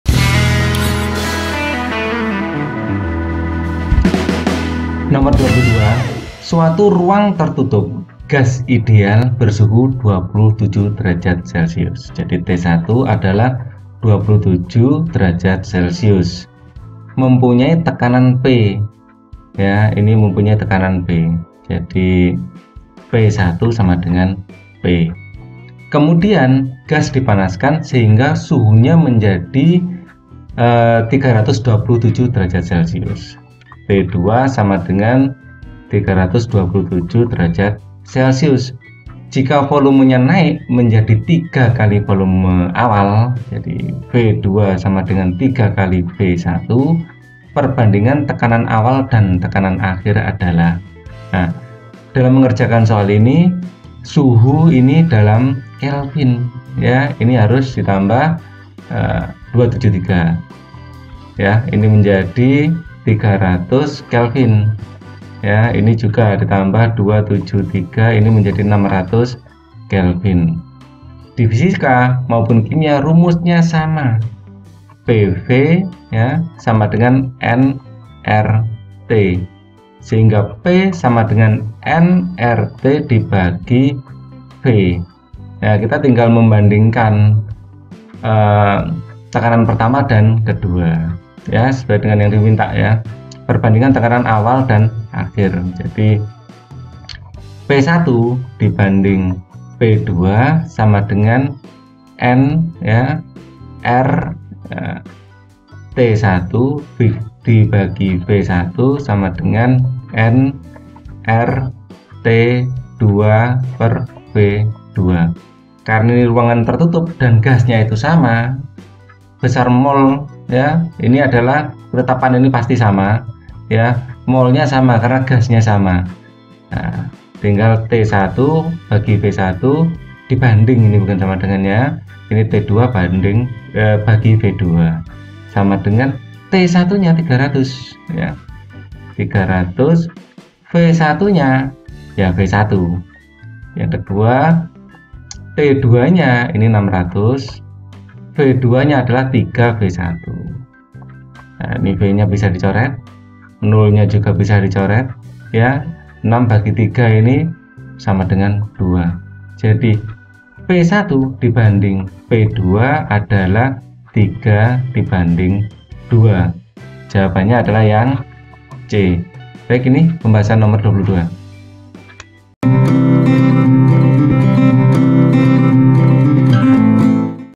perbandingan tekanan, gas ideal, fisika, un fisika 2016 no 22
22.‘ Suatu ruang tcrtutup gas ideal bersuhu 27 °C mcmpunyai tekanan P. Kcmudian gas
dipanaskan schingga suhunya menjadi 327 °C. Jika volumcnya naik mcnjadi tiga kali
volume awal, perbandingan tekanan awal clan akhir adalah

A. 1:4
B. 2:3
C. 3:2
1). 3:6